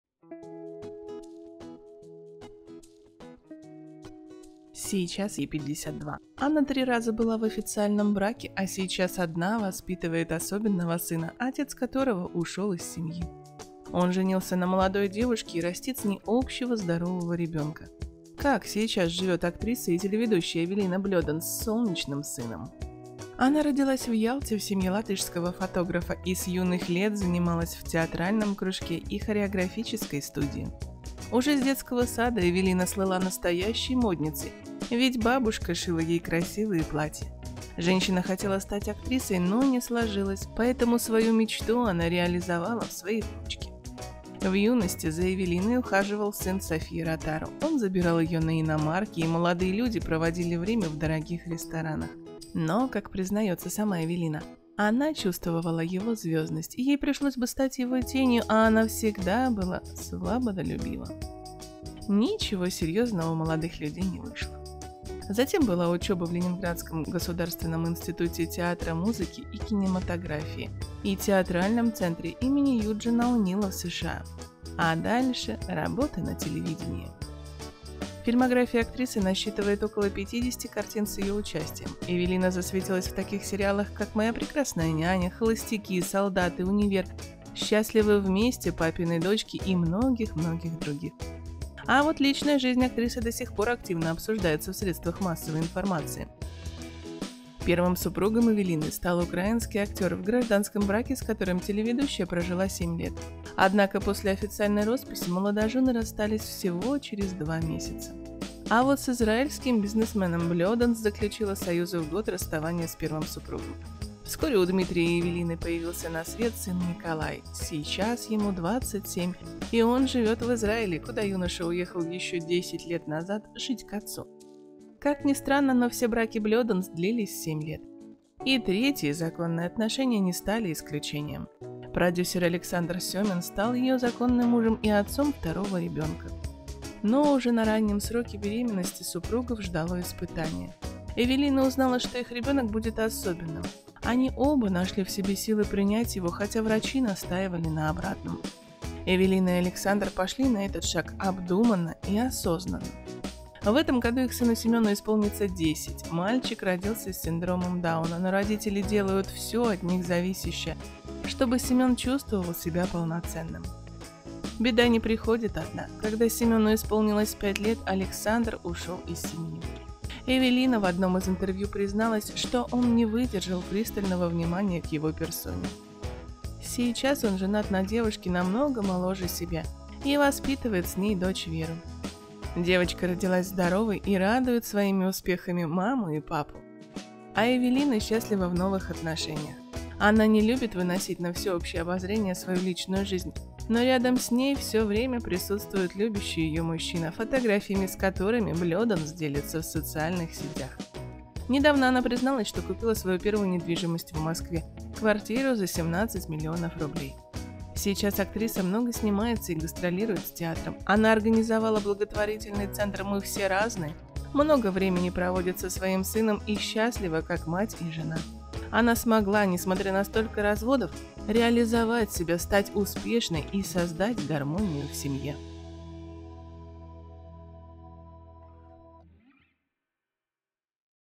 Как сейчас живет Эвелина Бледанс с "солнечным" сыном после развода с мужем?
Сейчас ей 52 года, она три раза была в официальном браке, а сейчас одна воспитывает особенного сына, отец которого ушел из семьи. Он женился на молодой девушке и растит с ней общего здорового ребенка. Как сейчас живет актриса и телеведущая Эвелина Бледанс с "солнечным" сыном?